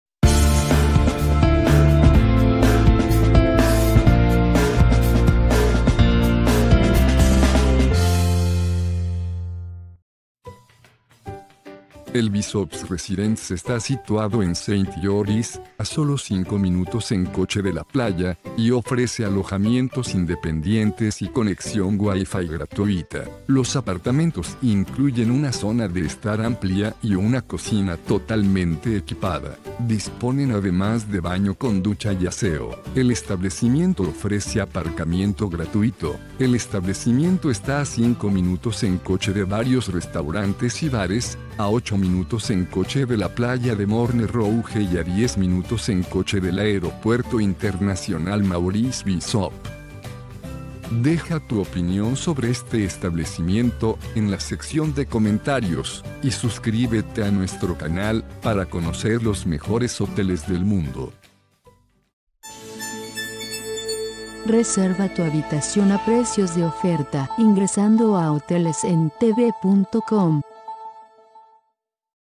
Bishops Residents, Apartamento en Saint Georgeʼs, Granada
Bishops Residents presenta Apartamento con servicio de habitaciones, habitaciones no fumadores y servicio de lavandería.